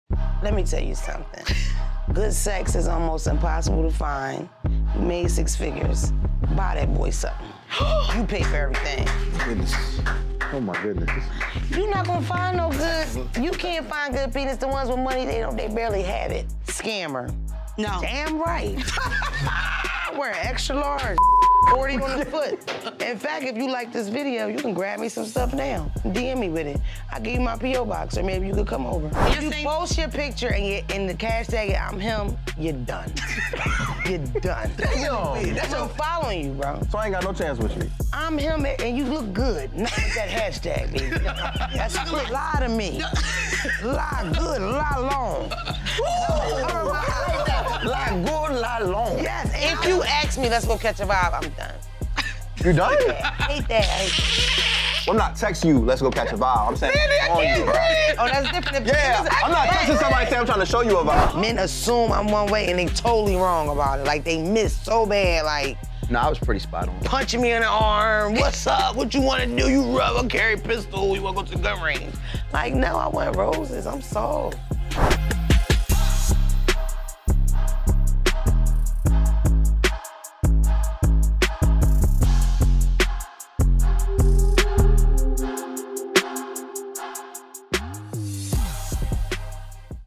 Is Mona Speaking Facts When It Comes To Compromise In Relationships? 😩
In honor of Love week, BET will be teaming up with Mouse Jones, Mona & Mandii B., some of the most toxic influencers in the game to talk dating, love, and red flags that signal you to get it OVER WITH!